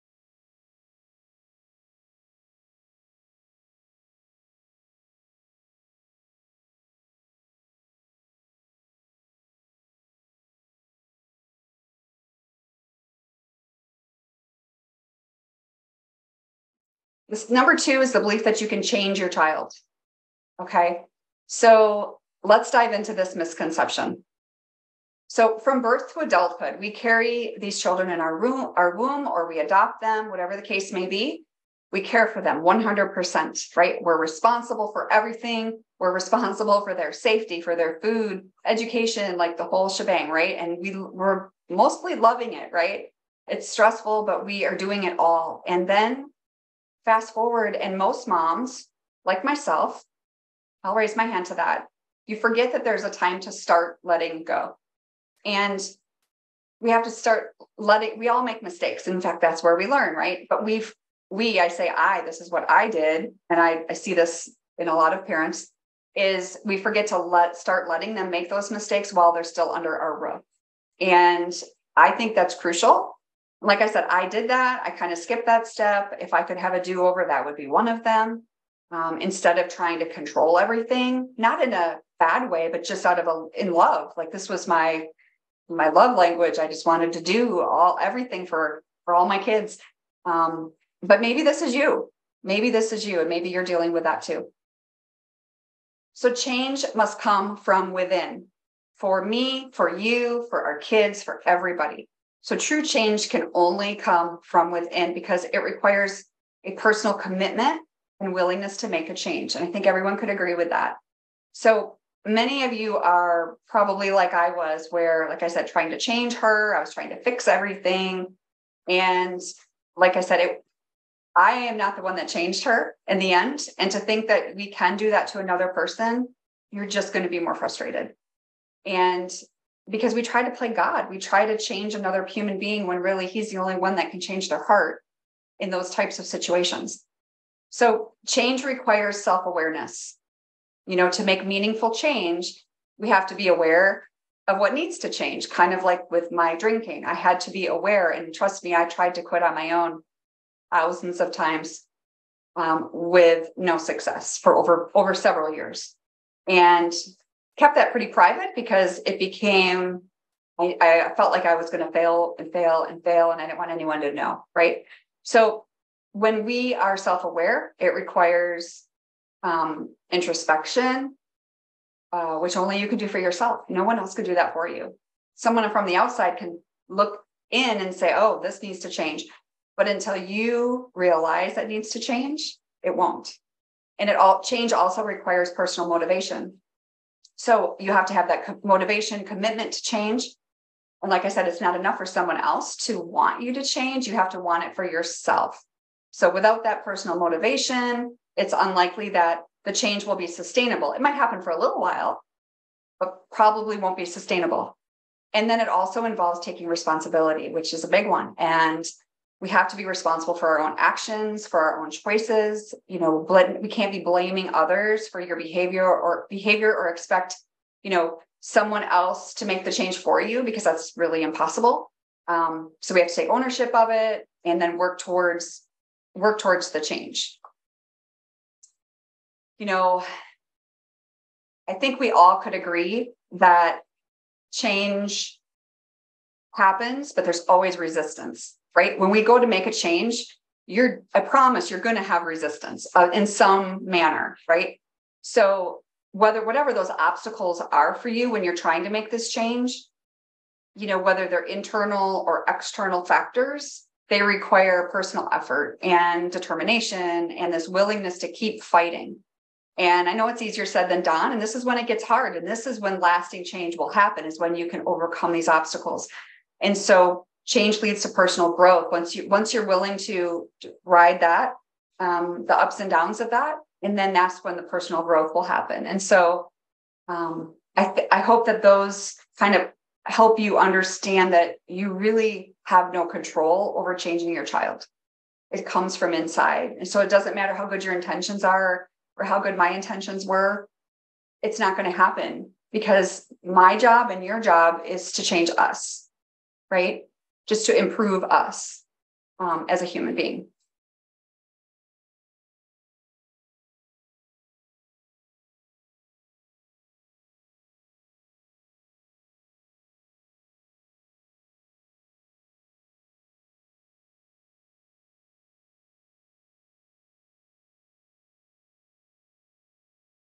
Sally Harris on How to Stop Letting Estrangement Control Your Life - How to Reclaim Control
Next on Families Divided, Sally Harris will discuss How to Stop Letting Estrangement Control Your Life - How to Reclaim Control and Find Peace.

Watch the premiere on December 27, 2023 at 9PM.